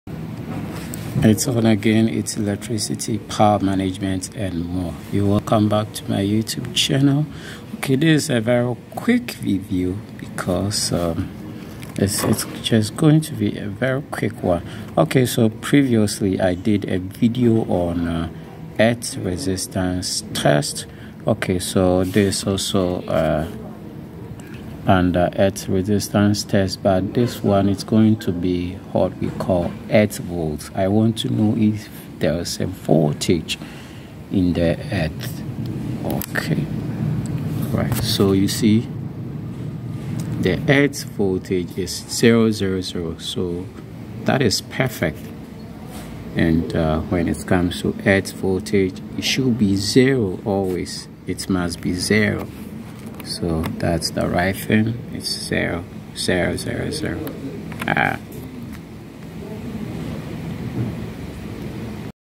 Building In Ghana | How To Test Earth Voltage
Earth or ground voltage test is done or run on earthing and grounding systems to know the voltage in the specific Earthing or grounding system. Earth voltage must be 0.00 volt.